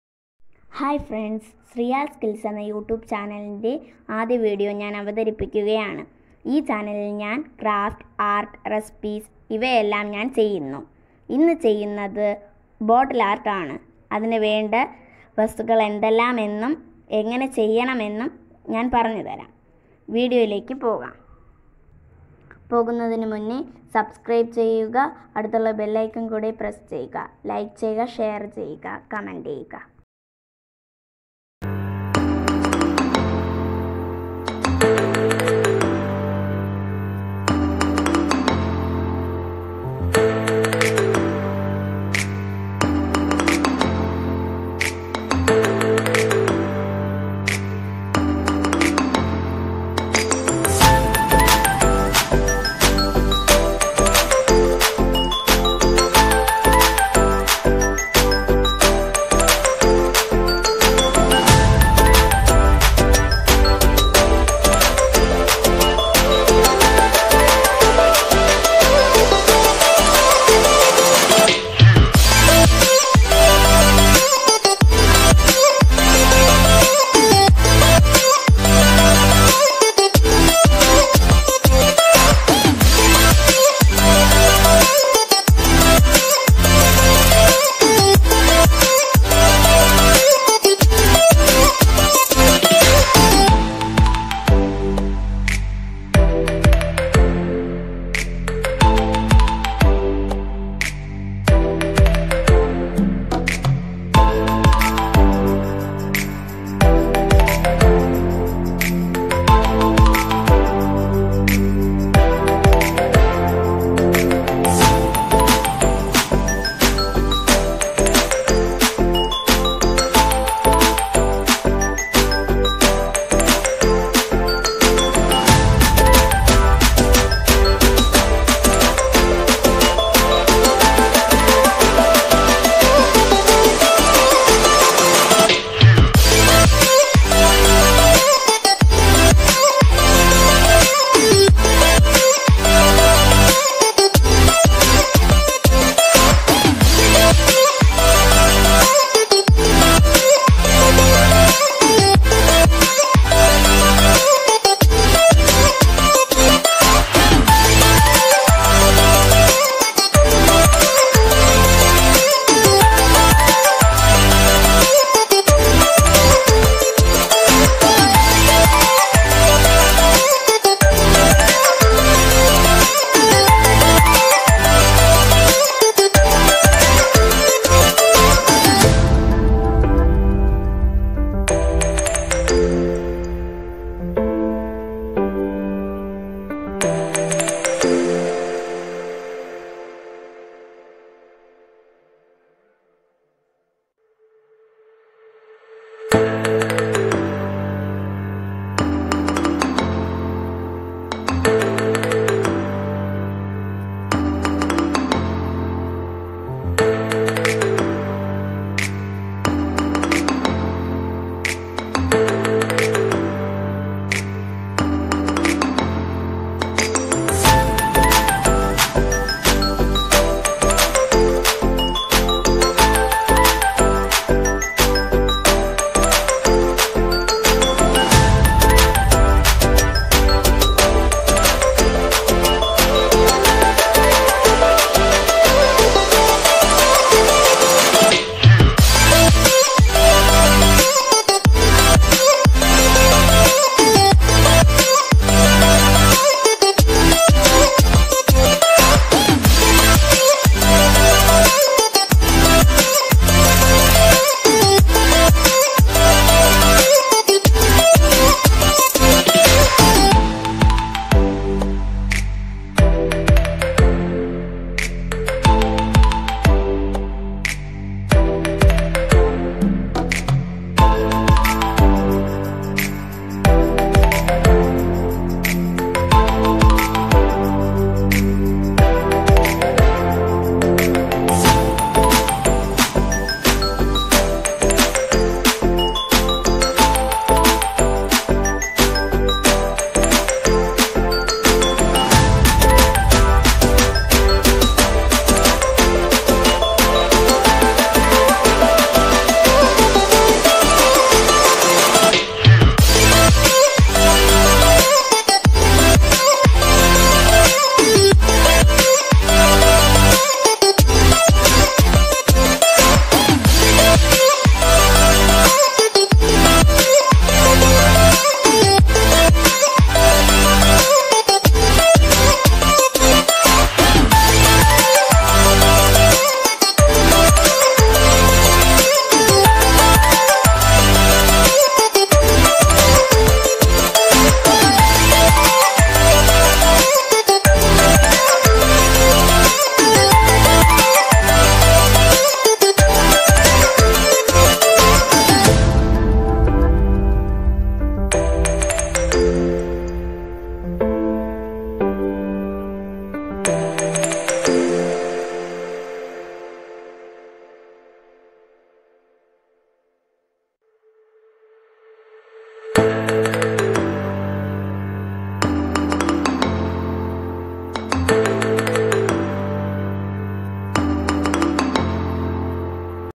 Easy Bottle Art for kids|Simple bottle painting
Easy Bottle Art using Bottle, woolen thread, fabricpaint, Paper